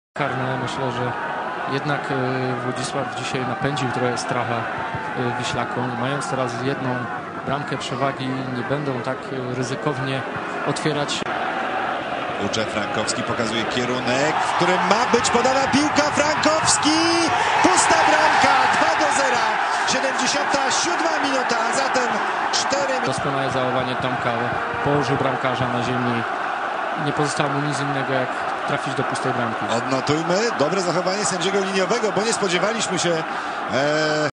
2004.05.16 - Wisła Kraków 2:1 Odra Wodzisław Śląski
Wisła Kraków 2-1  Odra Wodzisław Śląski 16 maja, 19:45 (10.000)
Tomasz Frankowski 74, 77 - Marek Kubisz 87